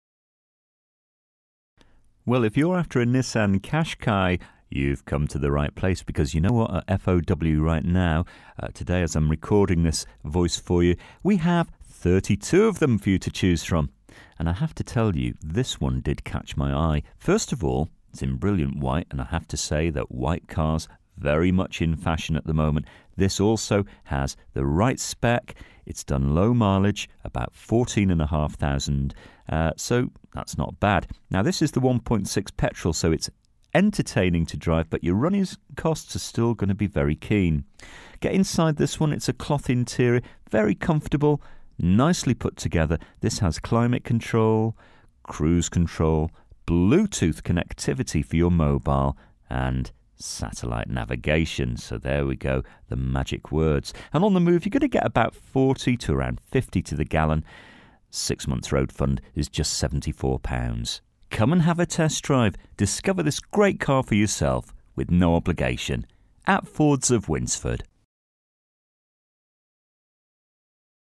NISSAN QASHQAI HATCHBACK (2011) 1.6 [117] N-TEC 5DR - BG11PWU
DETAILS:
First registered: 15/04/2011
Current mileage: 14420
Registration: BG11PWU
Colour: WHITE

FEATURES:
all round electric windows, remote central locking, cloth interior, all round airbags, climate control, pas, abs, alloy wheels, solid paint, cd, traction control, satellite navigation, parcel shelf present, hand book present, 2WD on 4x4 shape, cruise control, bluetoot

This used car is for sale at Fords of Winsford Car Supermarket in Cheshire, UK